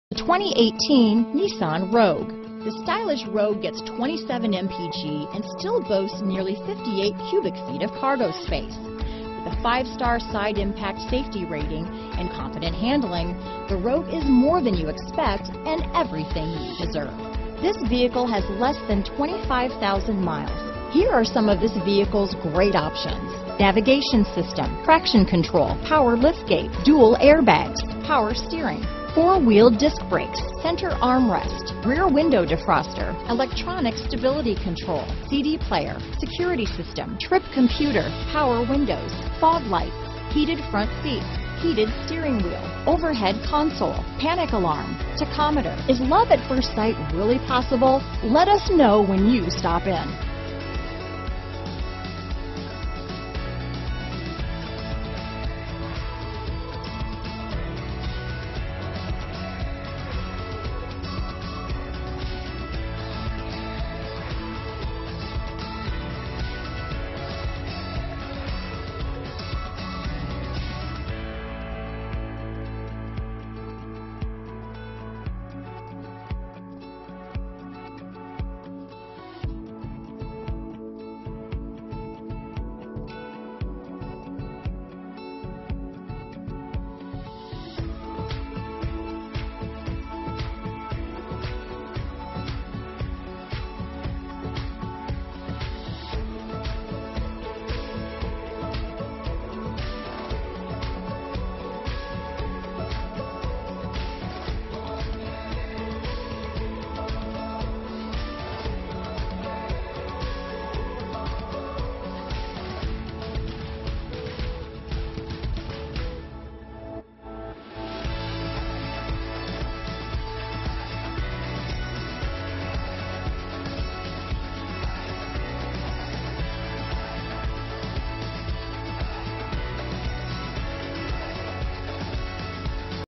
2018 Nissan Rogue Sayre, PA, Binghamton, Ithaca, NY, Scranton, PA, Endicott, NY TTP6426
Magnetic Black U 2018 Nissan Rogue available in Sayre, Pennsylvania at Williams Toyota Sayre. Servicing the Binghamton, Ithaca, NY, Scranton, PA, Endicott, NY area.

Recent Arrival! This 2018 Nissan Rogue SL in Magnetic Black features: Premium CARFAX One-Owner. Clean CARFAX. AWD CVT with Xtronic 2.5L I4 DOHC 16Vbrbr25/32 City/Highway MPGbrbrbrSee the rest of our inventory at WILLIAMSTOYOTA.COM.